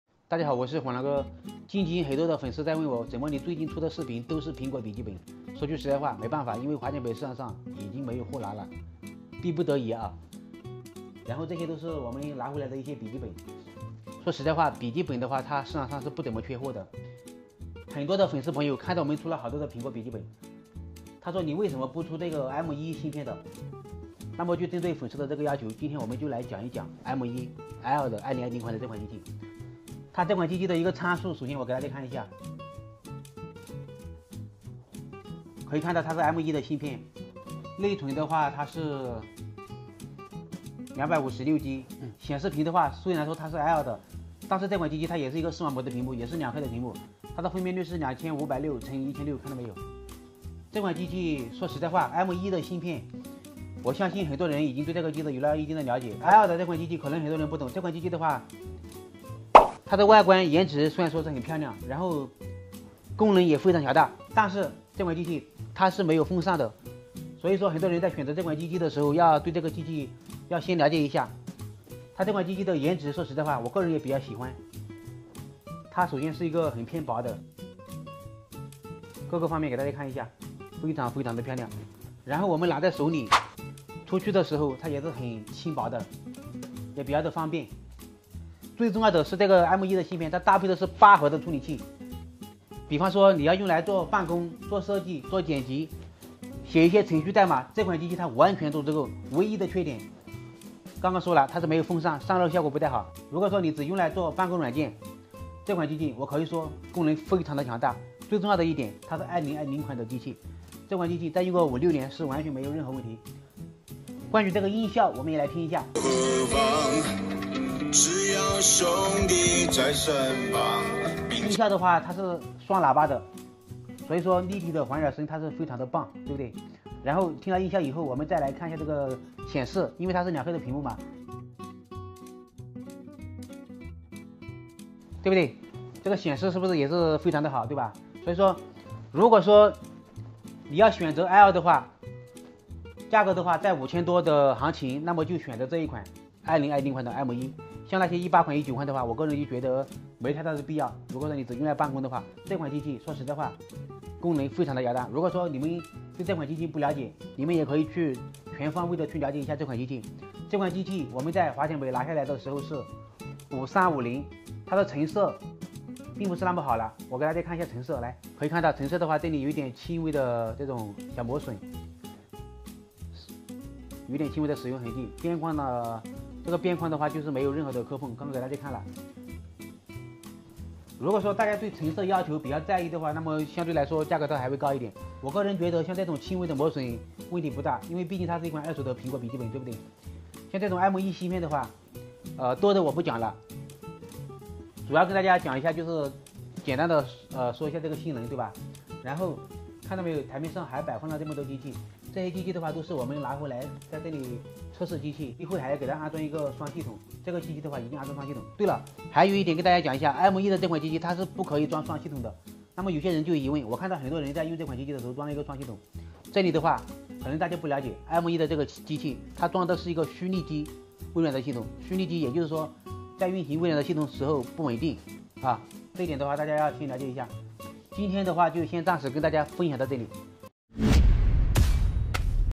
机皇苹果 MacBook air 如今跌大跌，分析 m1 处理器这价格果断拿下
極客 blog

水光震蕩療法
Water-Light Oscillation Theory Non-Drug Therapy